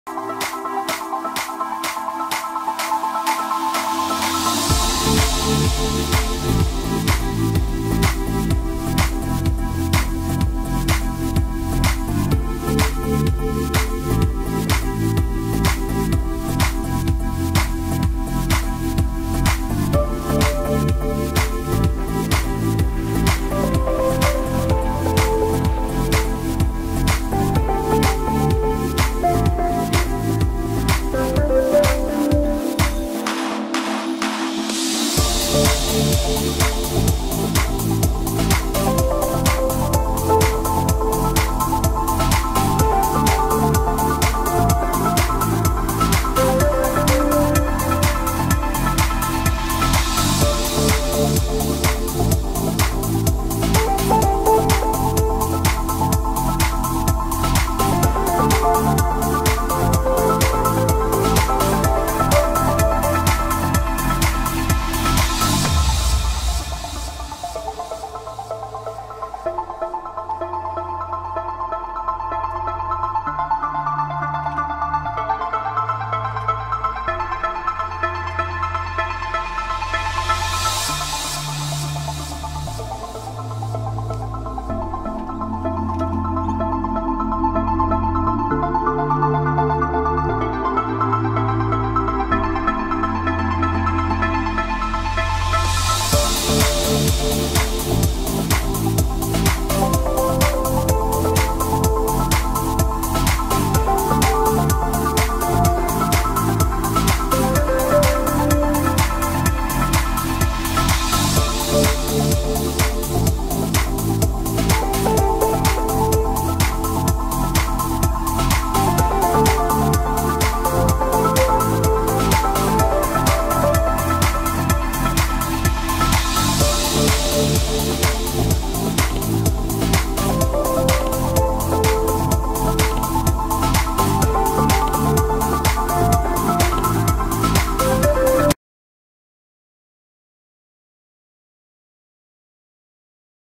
Wharram Tiki 26 Madgic Catamaran Sailing
The Wharram catamaran Tiki 26 Madgic sailing in Poole Harbour, UK